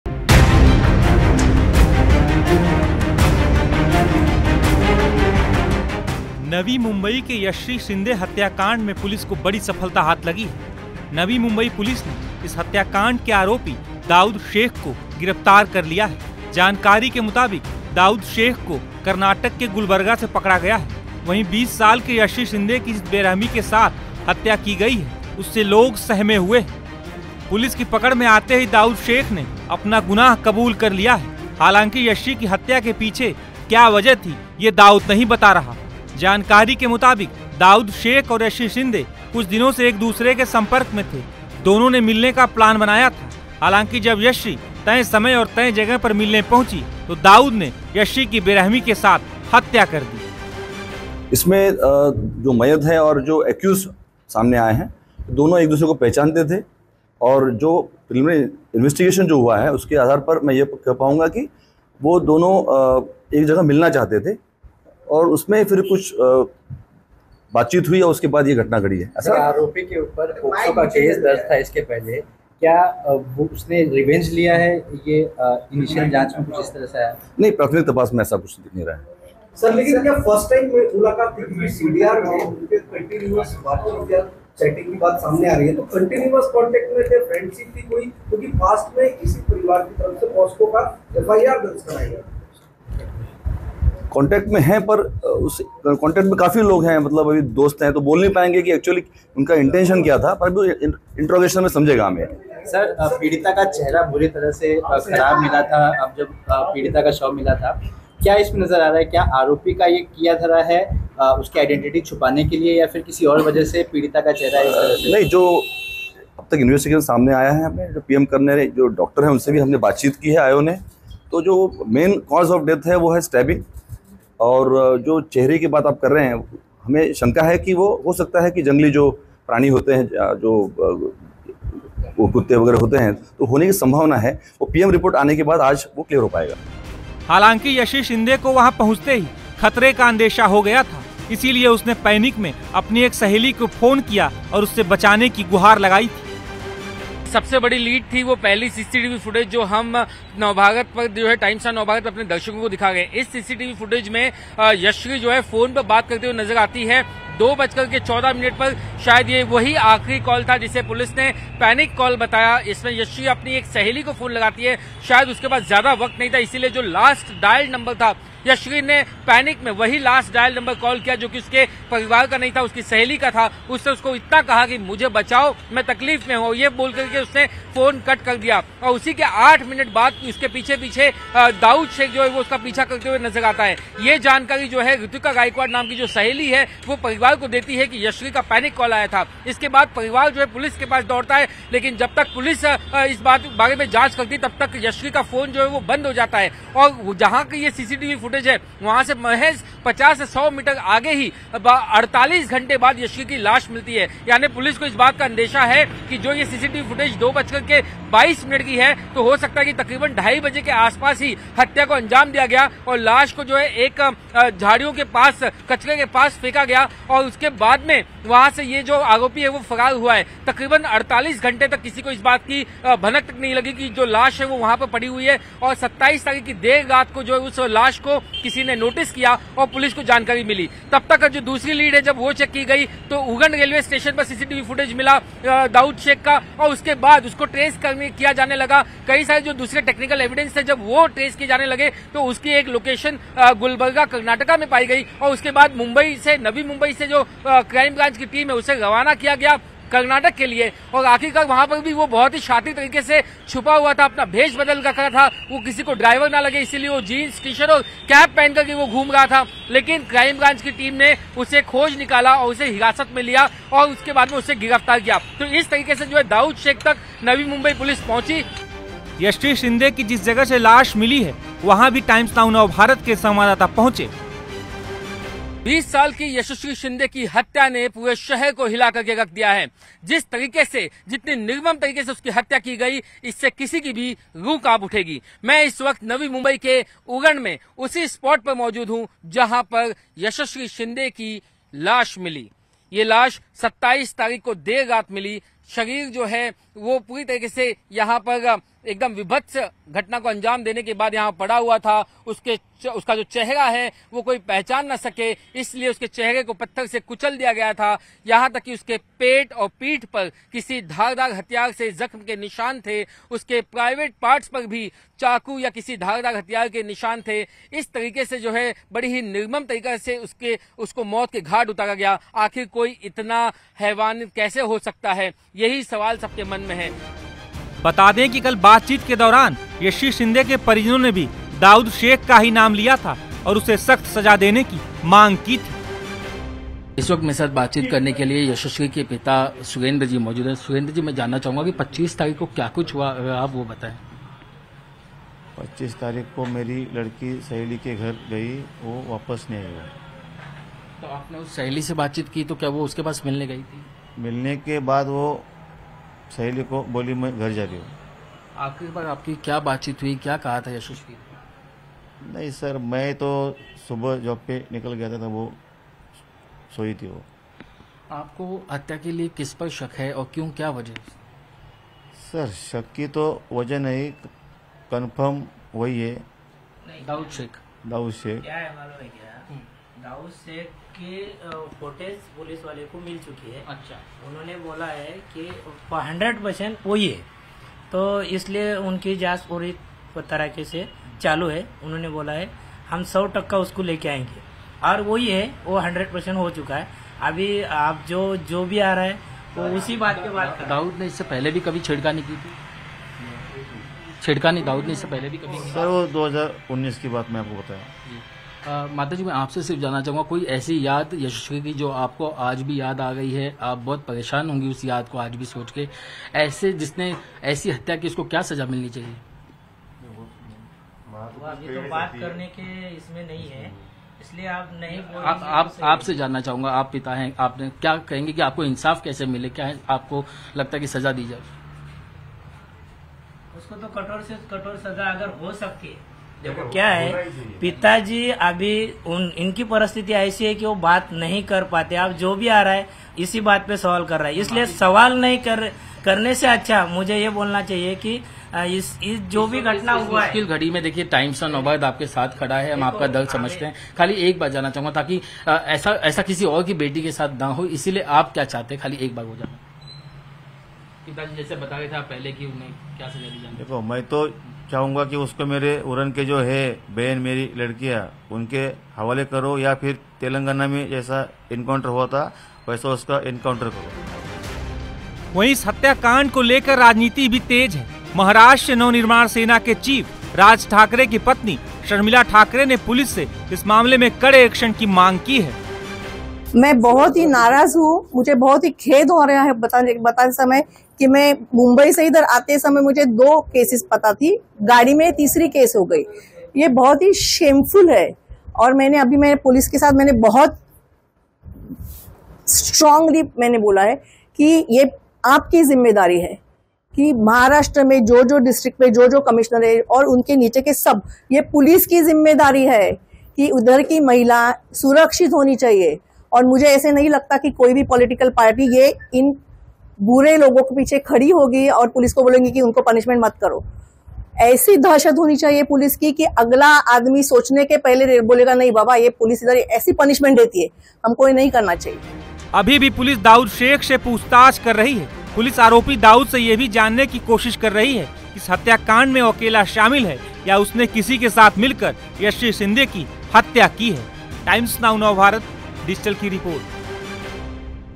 Yashashree Shinde Murder Case : पुलिस की पकड़ में आते ही क्या बोला Dawood Sheikh ? Hindi News
Navi Mumbai के Yashashri Shinde हत्याकांड में पुलिस को बड़ी सफलता हाथ लगी है। Navi Mumbai Police ने इस Murder Case के आरोपी Dawood Sheikh को गिरफ्तार कर लिया है। जानकारी के मुताबिक दाऊद शेख को कर्नाटक के गुलबर्गा से पकड़ा गया है। वहीं 20 साल की यशश्री शिंदे की जिस बेरहमी के साथ हत्या की गई उससे लोग सहमे हुए हैं।